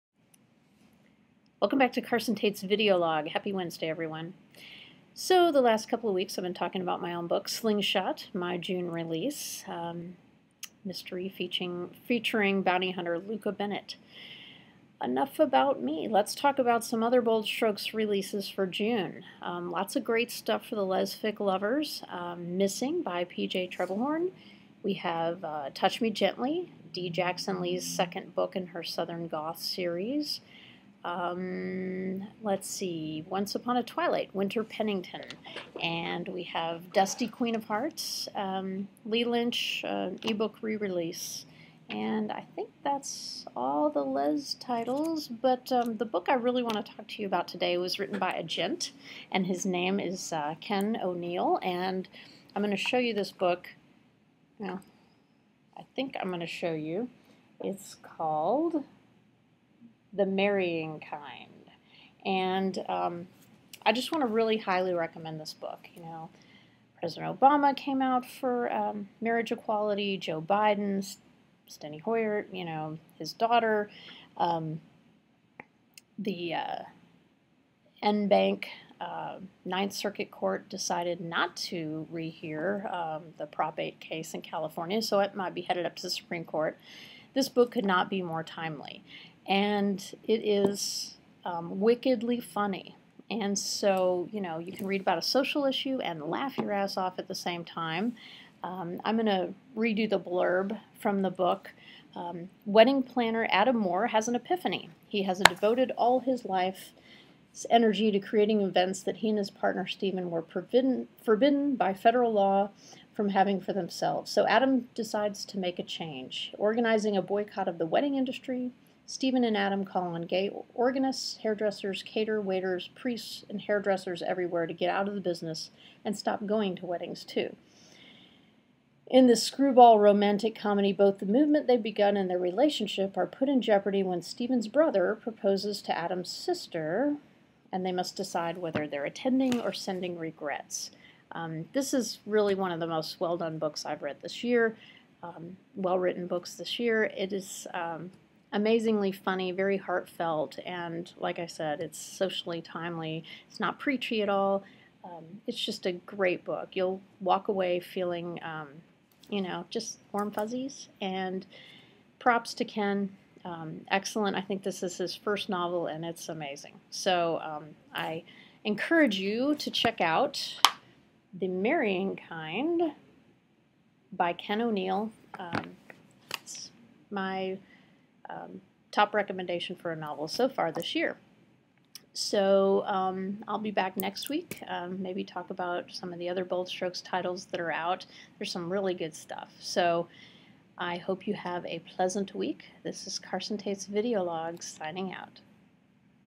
The Marrying Kind
Props to Bold Strokes Books author Ken O'Neill for his romantic comedy, The Marrying Kind. A must read for anyone who cares about marriage equality, romance, and likes to laugh out loud while reading.